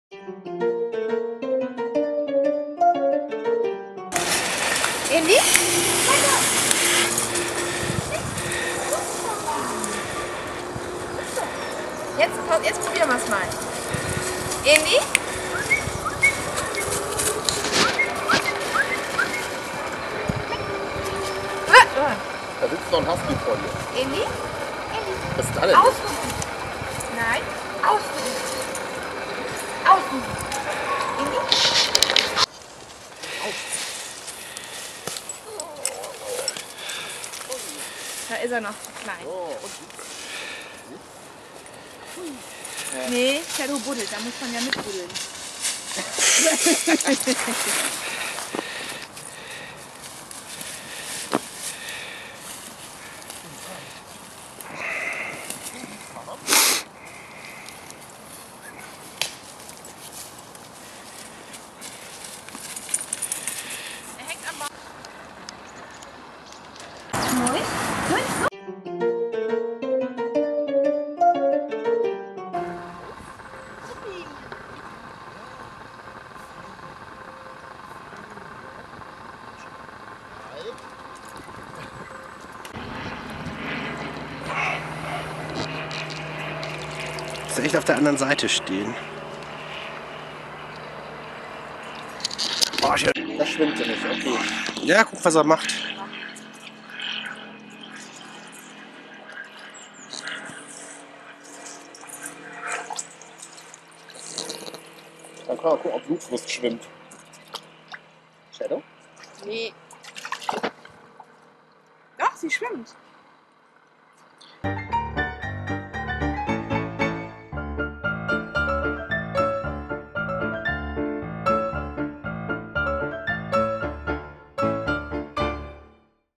Husky und Kurzhaar Collie spielen im Wald und am Bach
Erst ein bisschen Spielen im Wald und dann nicht über den Bach kommen. Aber beim "Nicht-drüber-kommen" haben Husky und Collie unterschiedliche Methoden.